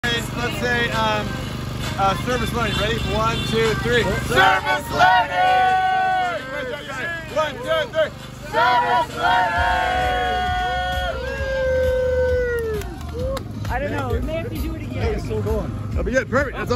Service Learning at Maui Oceanfront Marathon 5k Finish Line
ESRM 492 Service Learning in Maui on January 14, 2024. Rain was coming in later this morning so a group of us ran the 5k component of the Maui Oceanfront Marathon. This was at the finish line at Launiupoko Beach Park on Highway 30.

From left to right: Angelia, Carson, Rob, Dylan, Dr. A, Paula, and Simon.